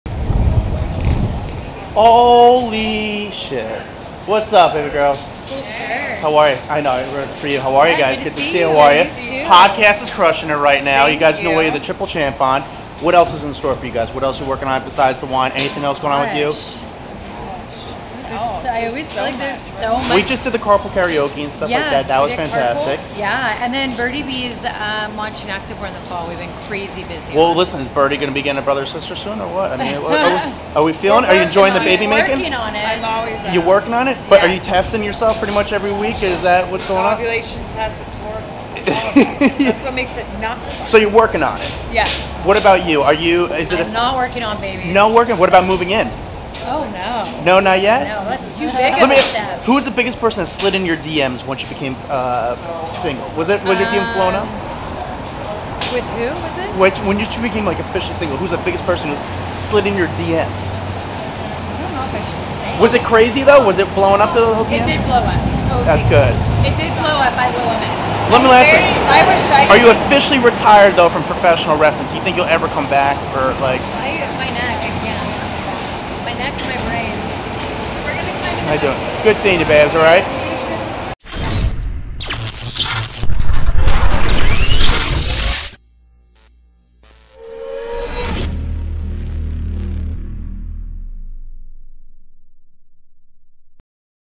Bella Twins: Nikki Bella says she's retired, Brie Bella wants another baby
While in New York City, Nikki Bella confirmed that she is retired because of a cyst on her brain and neck issues, Brie Bella is trying to have another baby with husband Daniel Bryan